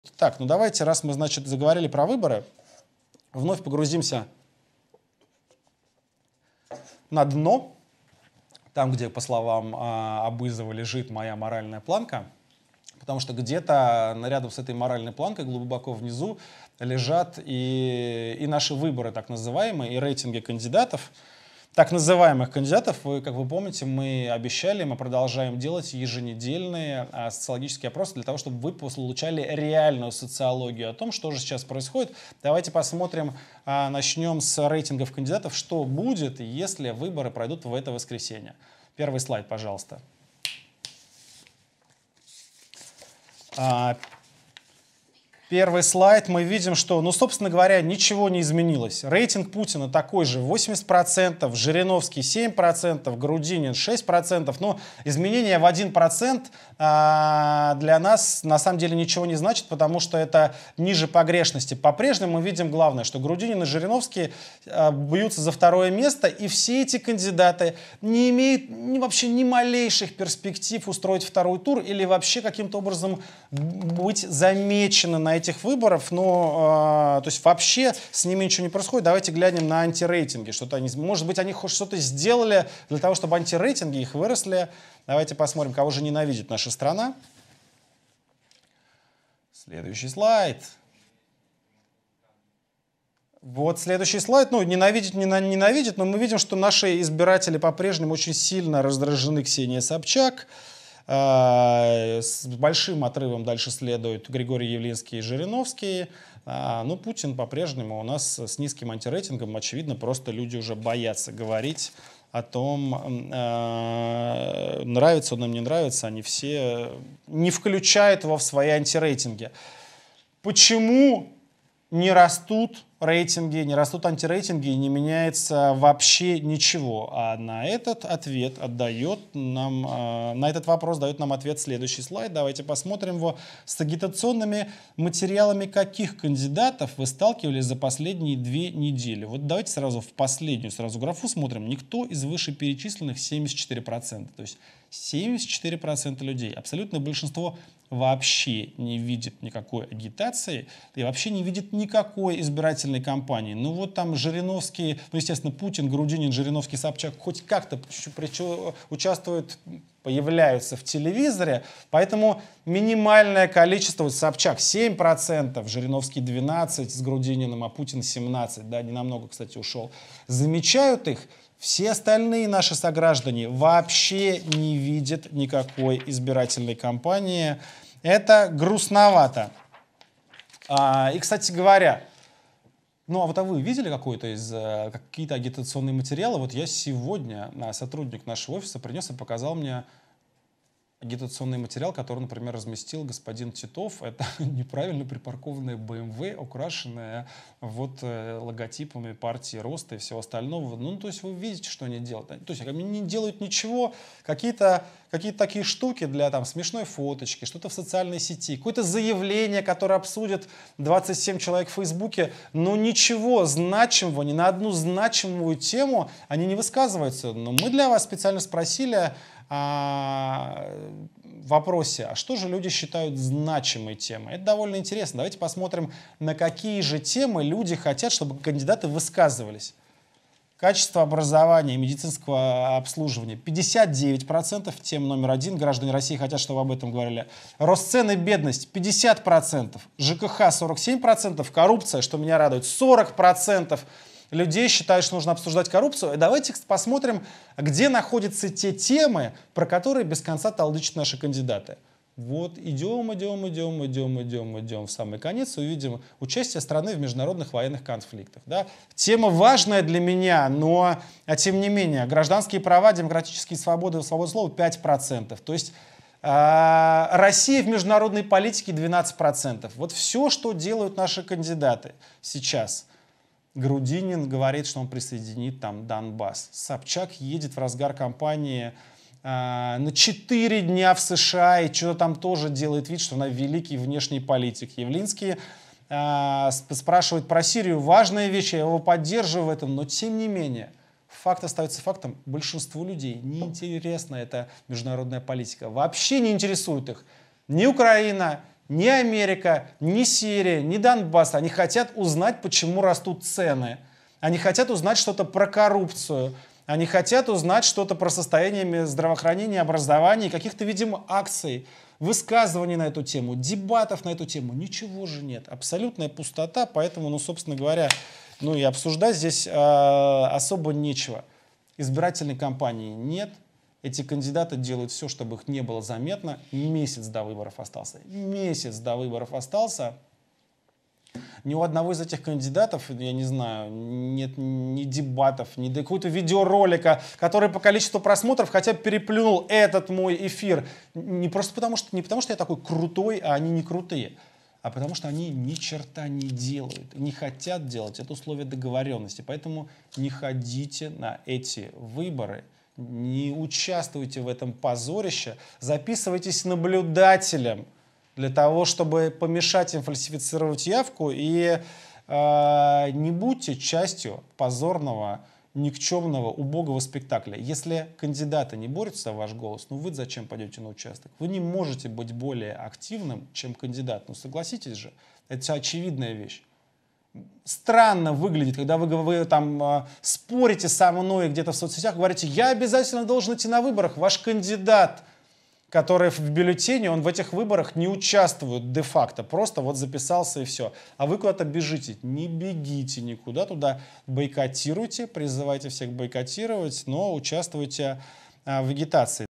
Что, если бы выборы президента проходили вчера (18 февраля)? Рассказывает Навальный
🌃 Что, если бы выборы президента проходили в это воскресенье (18 февраля 2018г.)? Или рейтинги кандидатов - результатами социологического опроса этой недели делится Алексей Навальный